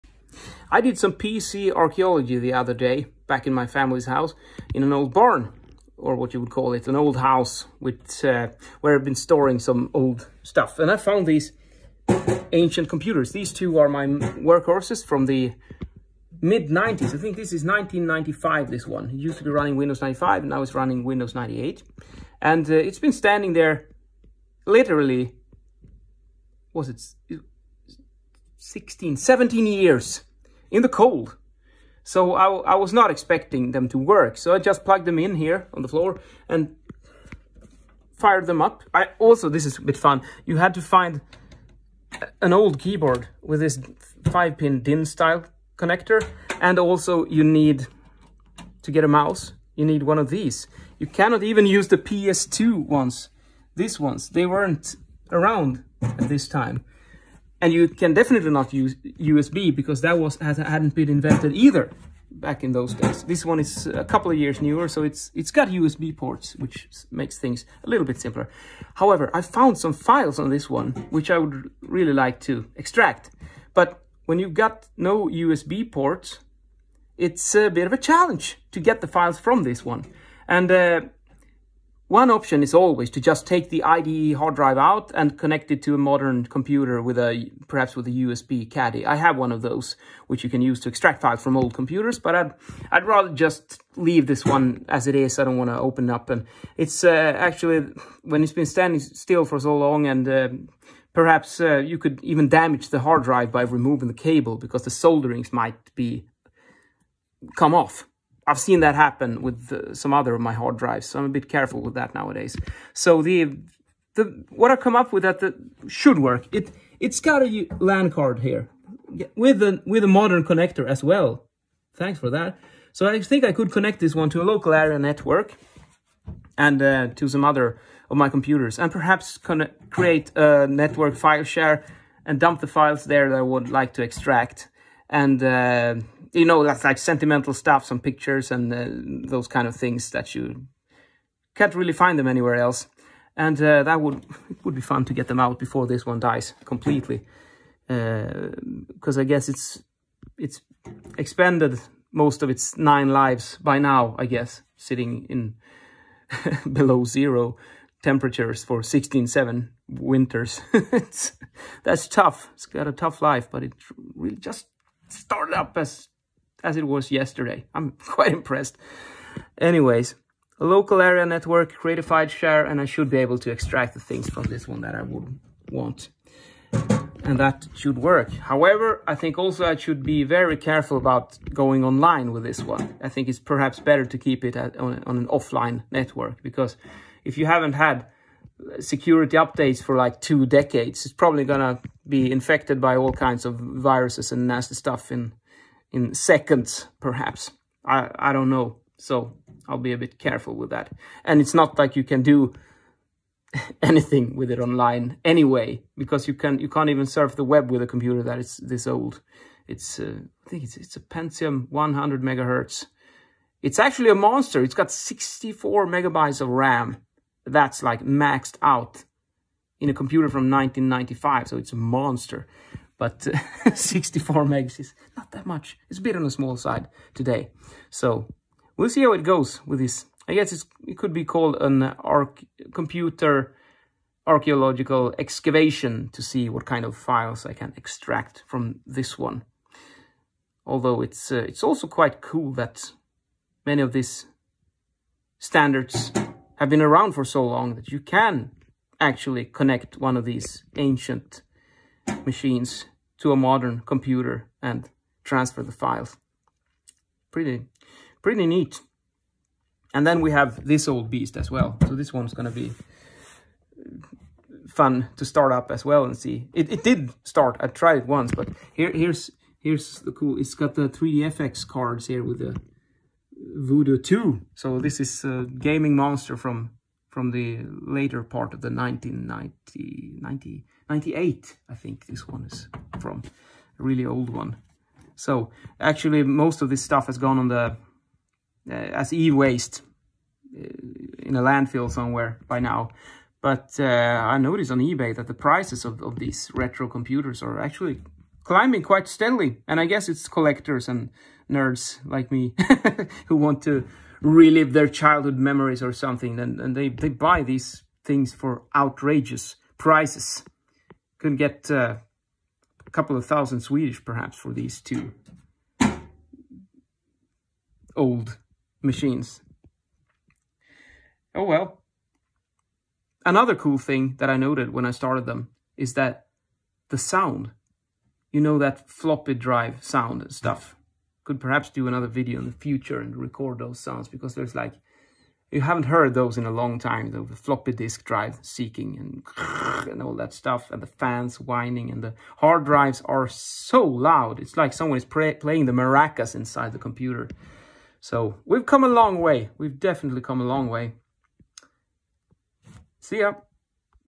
How to safely extract files from a retro pc from 1995?
I think a LAN file share will be my best bet, because it hasn’t got any USB ports. Pentium 100, 64 EDO RAM, Win98.